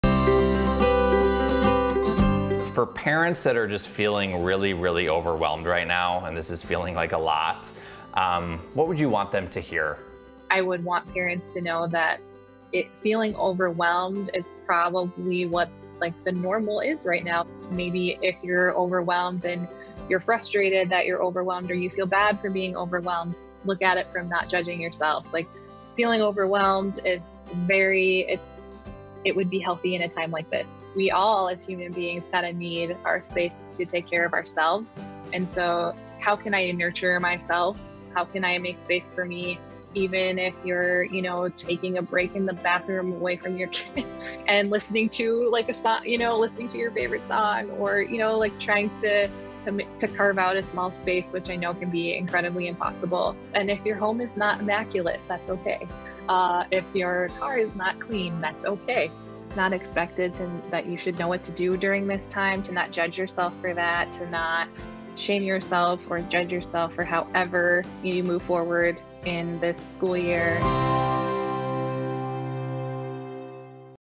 Pause for Parents: It's normal to feel overwhelmed right now
Licensed Clinical Social Worker Stephanie Jensen offers advice for parents as they and their families prepare for a new school year.